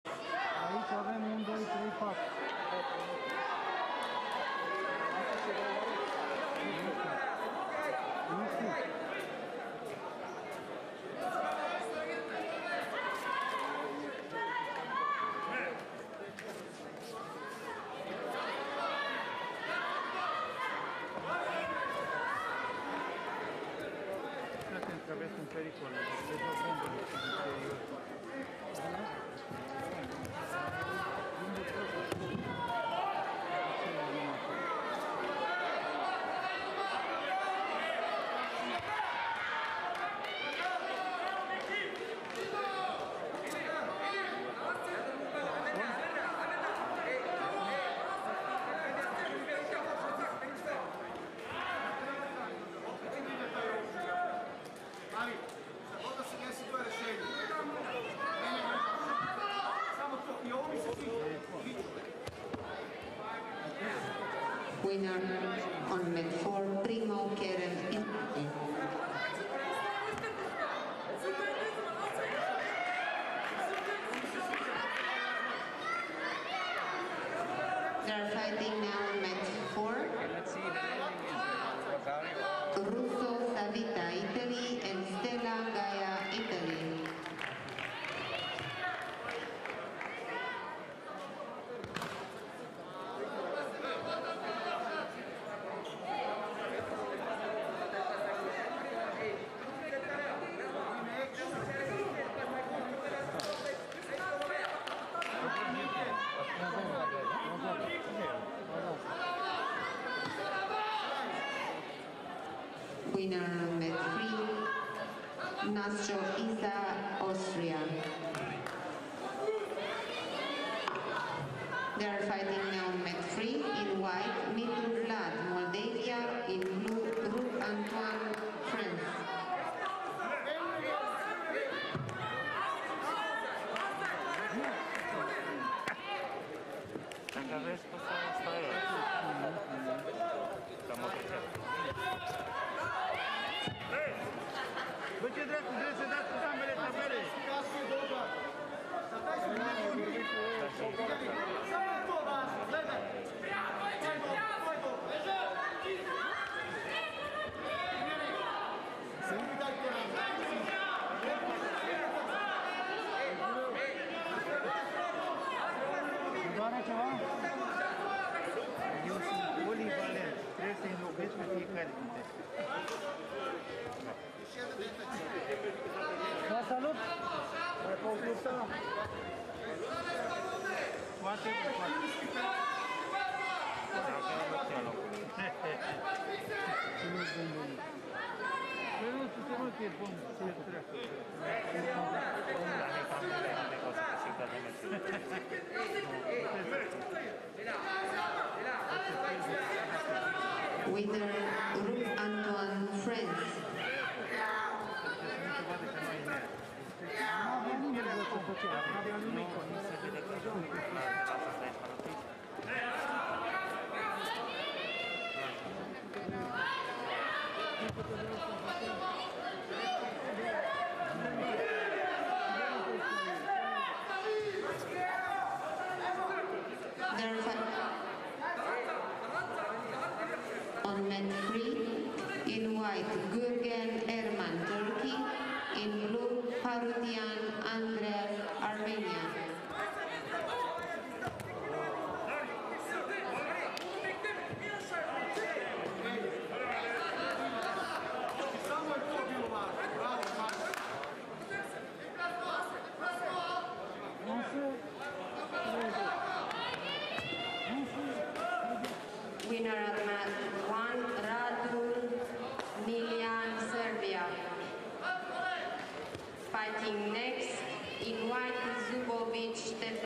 81.37 RADULJ Miljan (SRB) - GRIBINET Ciprian (MDA) Cadet European Judo Cup Bucharest Romania 2021
2. ДЕНЬ-1 , ТАТАМИ-1/2 Первенство Молдовы среди кадетов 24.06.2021 (фото поединка с ссылкой на YouTube)
1. НА САЙТЕ (Judo.MD) ПОЕДИНКИ.